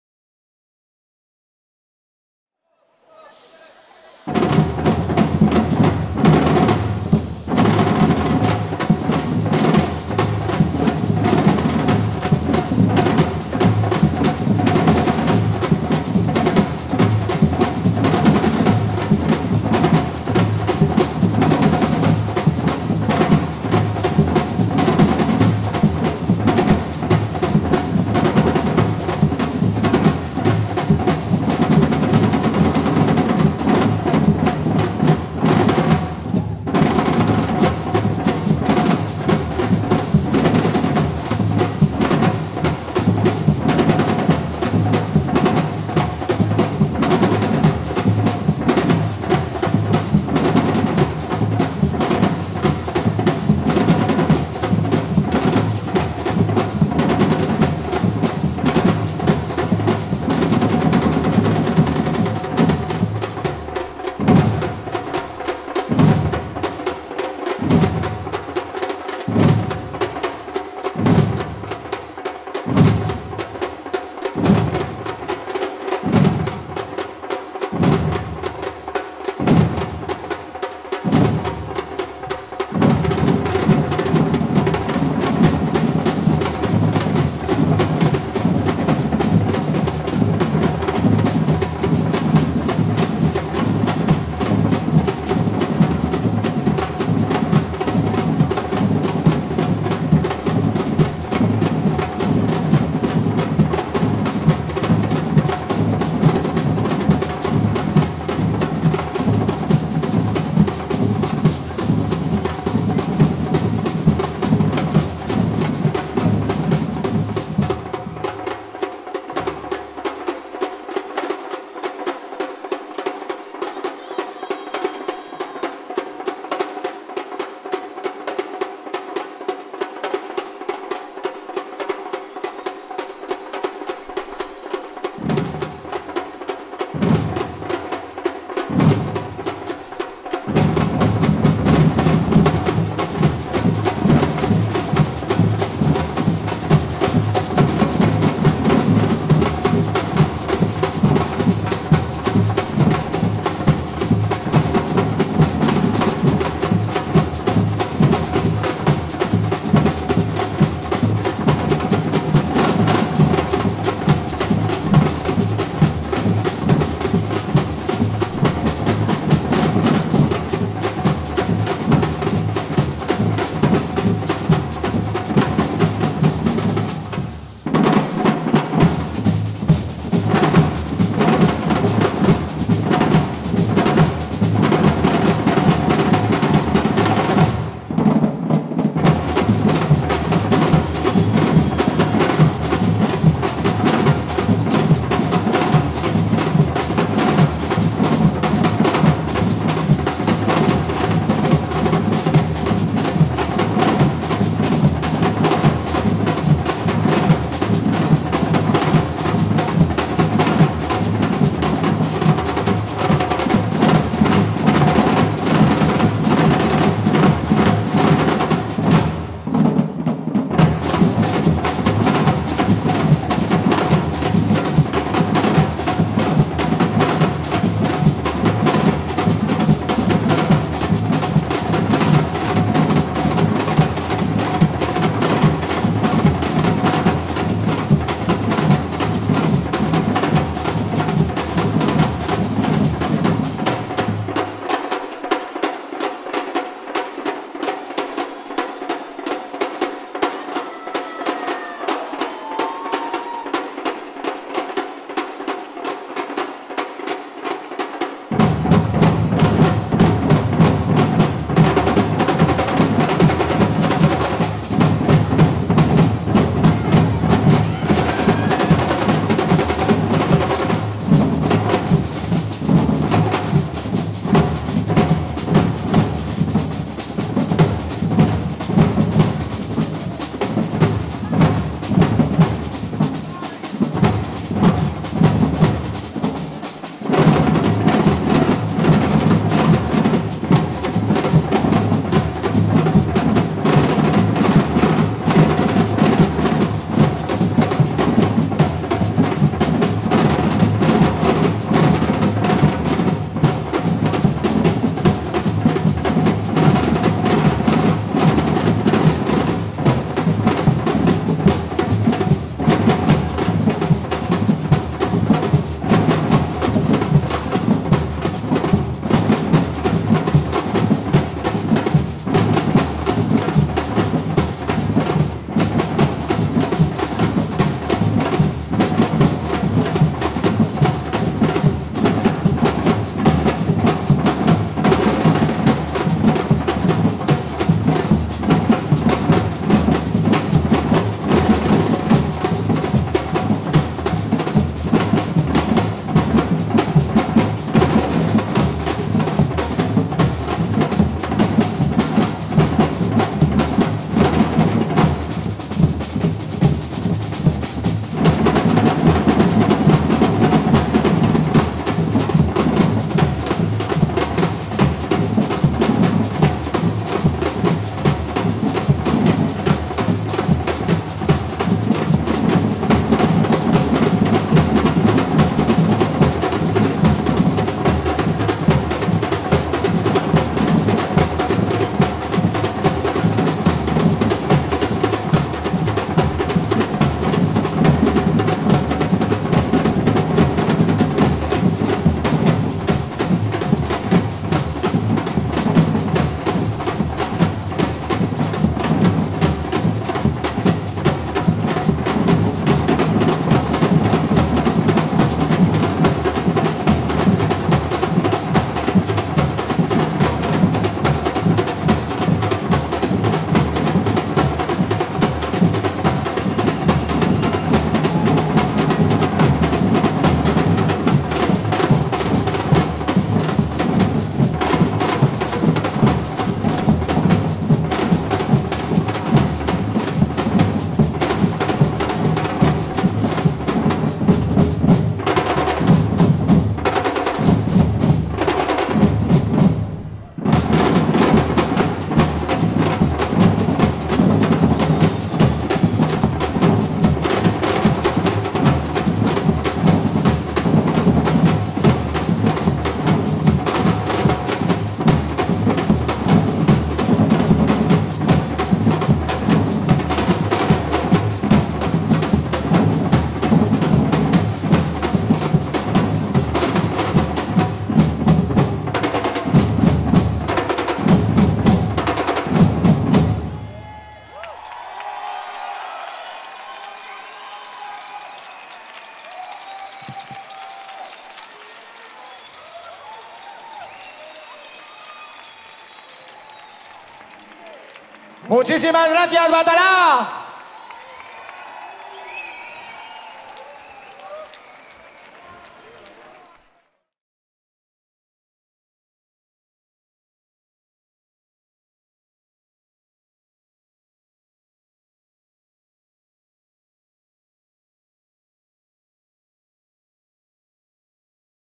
Concurso de batucadas Percumon 2019- Batalá Barcelona
Actuación de Batalá Barcelona en el teatro romano de Sagunto. Percumon 2019